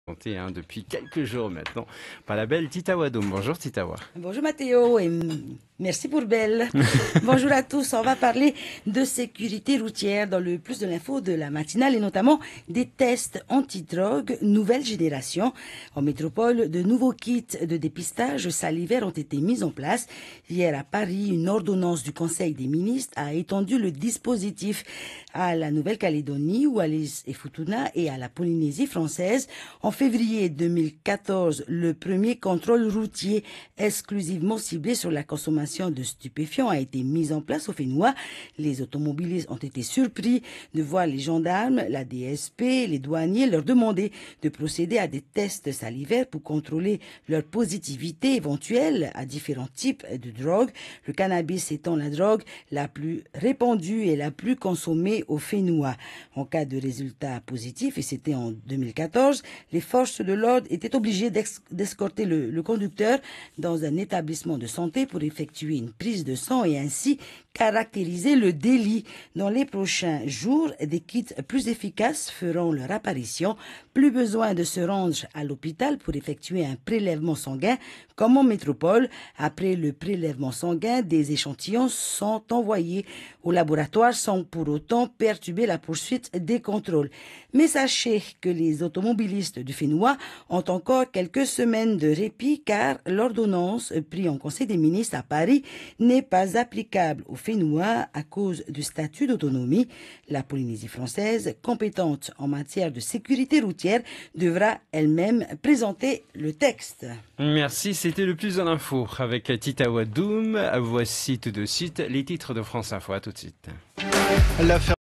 Le plus de l'info : nouveaux kits de dépistage salivaires - 06/04/2017
Le plus de l’info 6 avril 2017 a parlé  de « sécurité routière » dans le Plus de l’info de la matinale et notamment des tests antidrogue nouvelle génération.